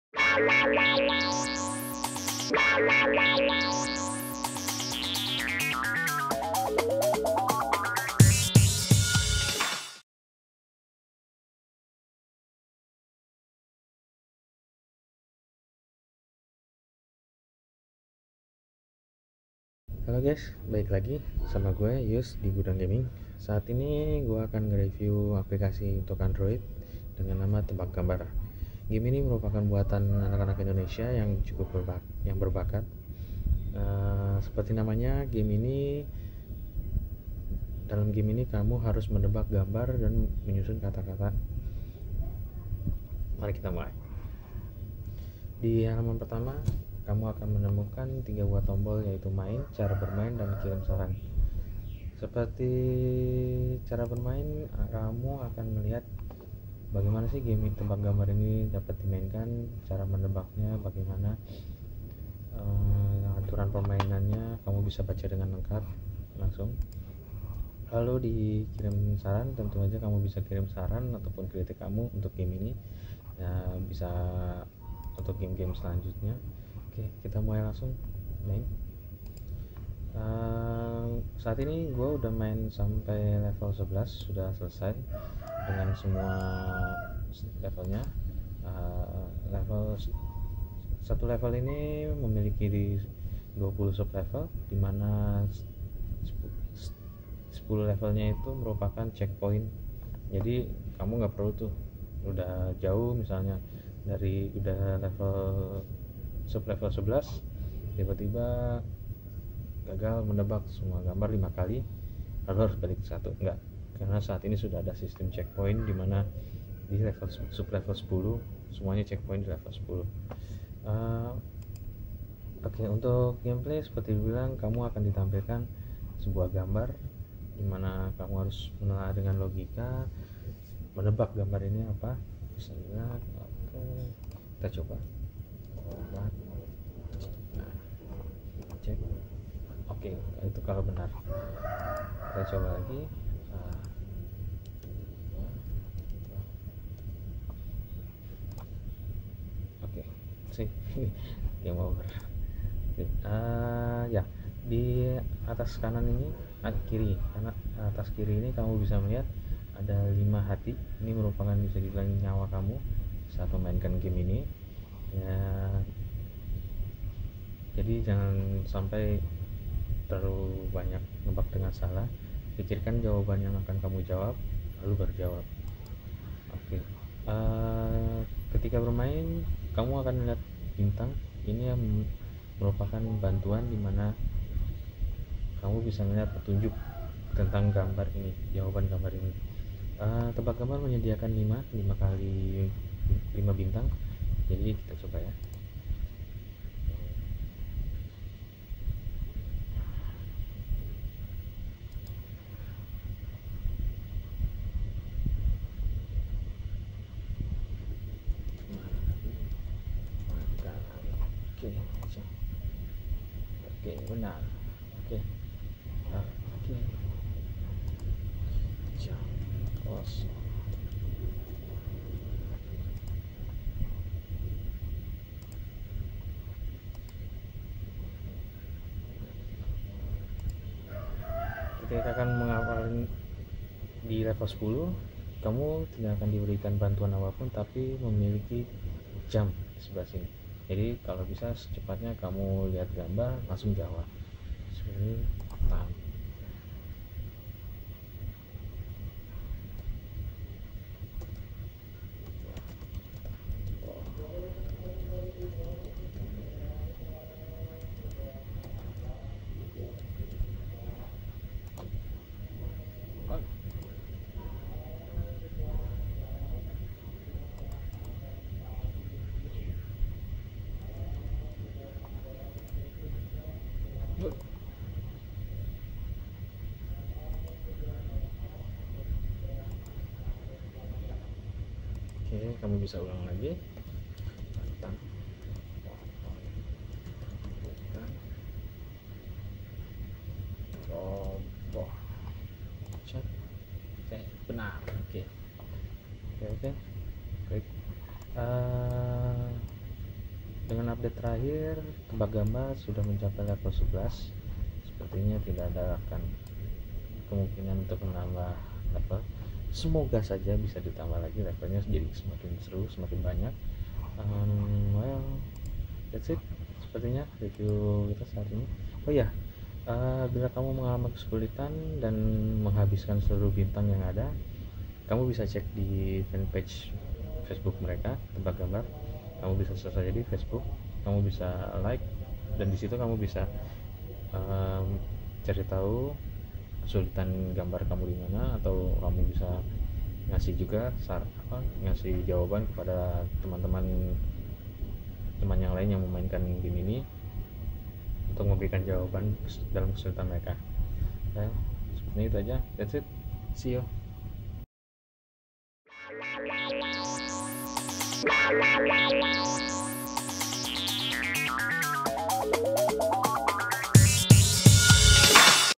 [GudangGaming] Review Game Android Tebak Gambar
Video Guide for Game's

Tebak Gambar merupakan game buatan anak bangsa Indonesia. Berawal dari sebuah ide sederhana, game Tebak Gambar ini langsung menarik bagi banyak gamer baik muda ataupun tua untuk mengasah otak masing-masing